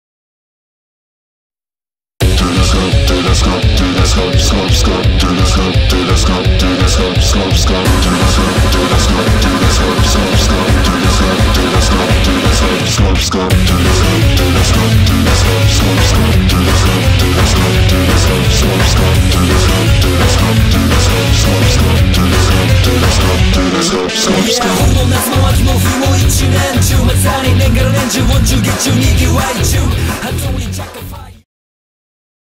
スコップグループPV（スコップ三島本店）
スコップ三島本店
名物の赤ホルモン、生キムチ、せせりが美味しい七輪炭焼の店スコップグループのPVが完成しました！